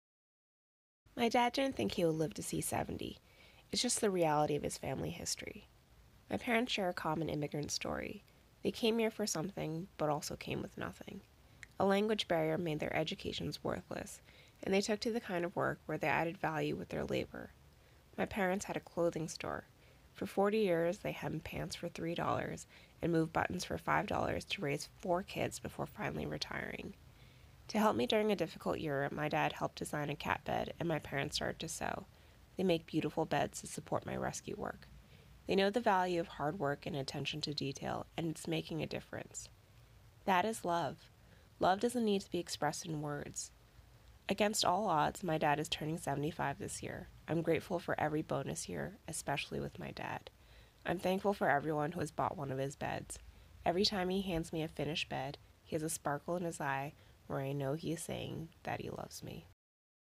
Cat Beds by CatDadHa & CatMomHa
The story and inspiration behind the cat beds made by my parents, affectionately known online as Cat Dad Ha and Cat Mom Ha.